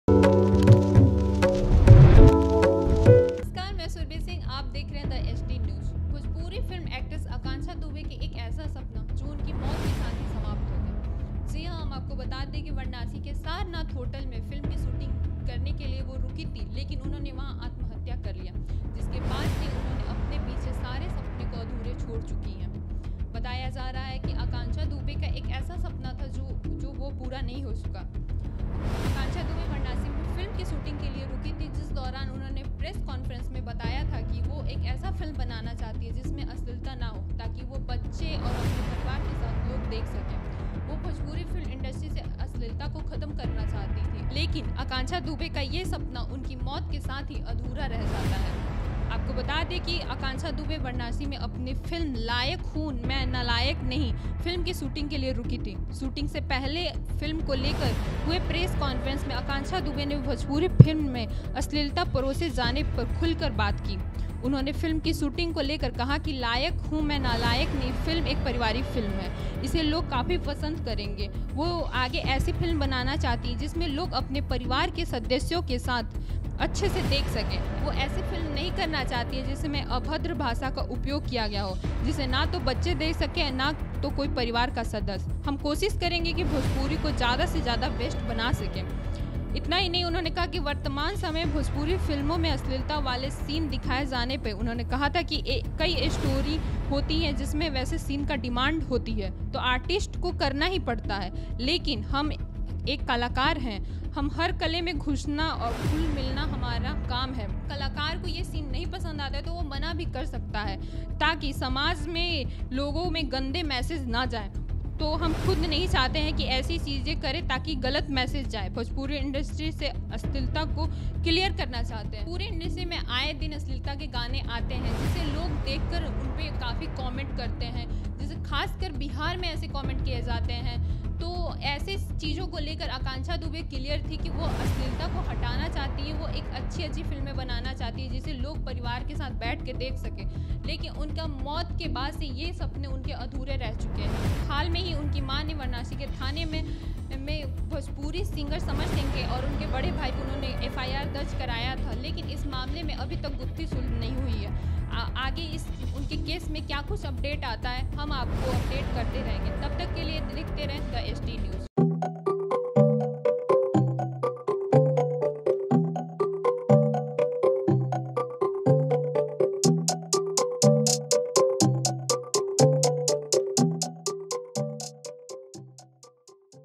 Varanasi: Actress Akanksha Dubey की मौत के रहस्य के साथ खत्म हो गई वो मुहिम, देखिए ये Report
Produced By: THE HD NEWS
Editor&GraphicsDesigner : Akash kumar
VideoEditor: Ravi kumar